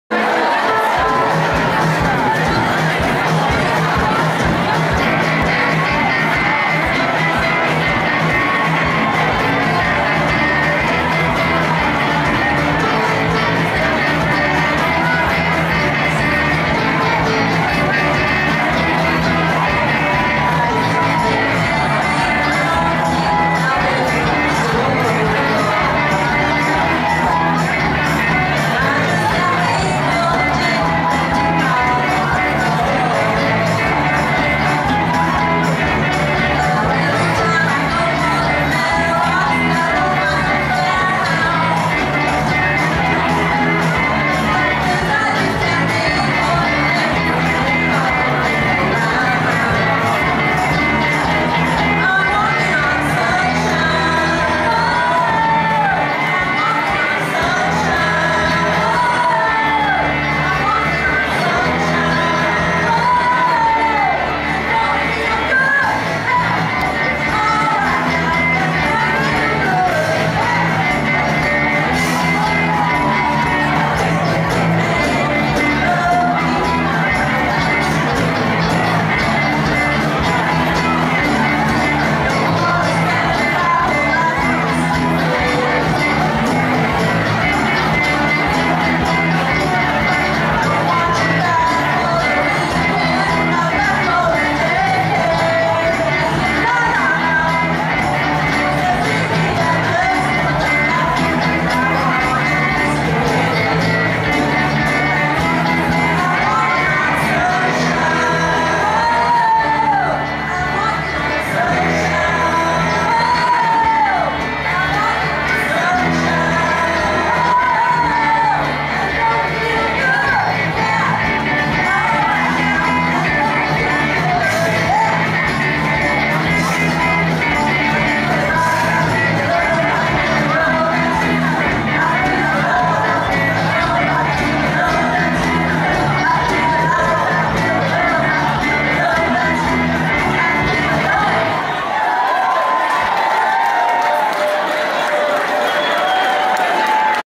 Central Commerce 14 Pub Night May 4, 2012.mpg
Band comprised of current and former teachers provided great music and added to the great fun  at the kick off for the 100th anniversary celebrations of Central Commerce May 4, 2012. All in total 500 people. In addition to celebrating these events will provide a scholardhip fund for current students. Great way of the alumni helping the current students.